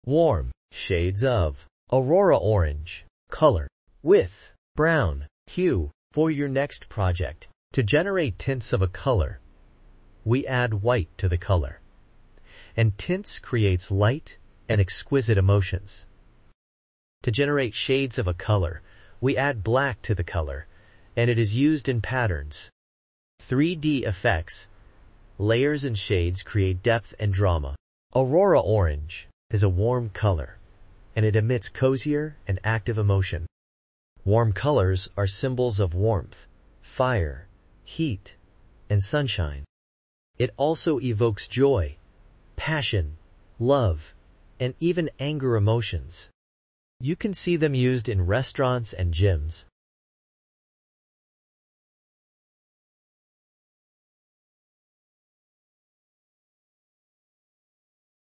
Shades & Tints of Aurora Orange color ec7042 A Warm Brown color
Shades of Aurora Orange color ec7042 Warm Shades of Aurora Orange color with Brown hue for your project!

Shades
Hex d1633a

Tints
Hex ee7f57

Aurora Orange is a warm color, and it emits cozier and active emotion. Warm colors are symbols of warmth, fire, heat, and sunshine. It also evokes joy, passion, love, and even anger emotions. You can see them used in Restaurants and Gyms.